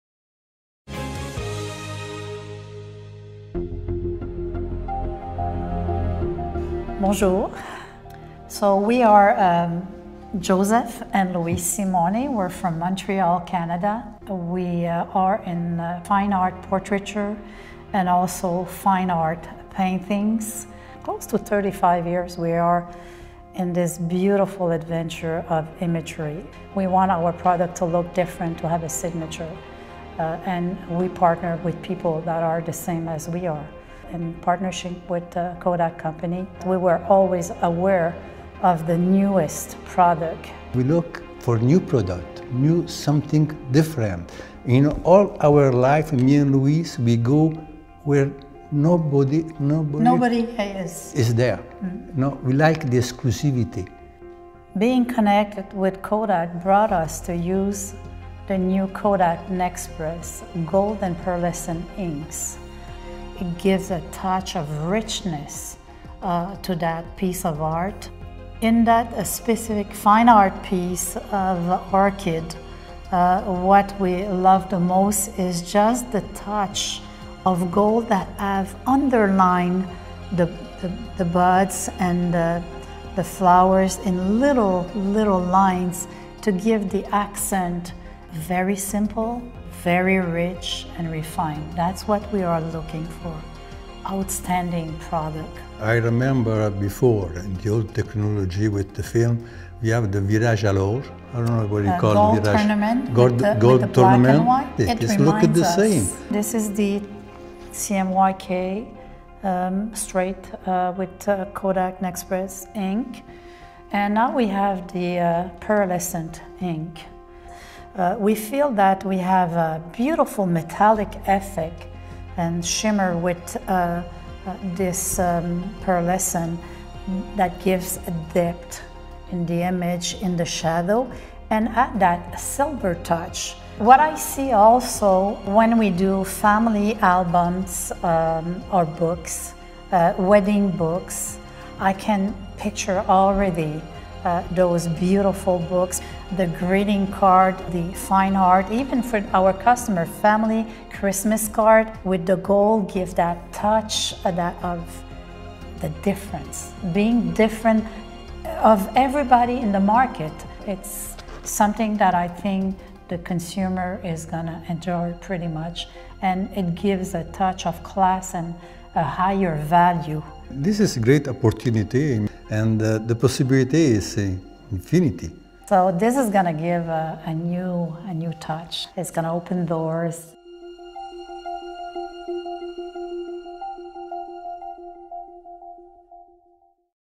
Simone's Signature Style with Kodak NexPress Gold and Pearlescent Dry Inks
Beautiful paintings with a difference.

The signature style of Louise and Joseph Simone, is further enhanced with the Kodak NexPress Gold and Pearlescent Fifth Imaing Unit Solutions. See how these new features enhance their portraiture and fine art prints.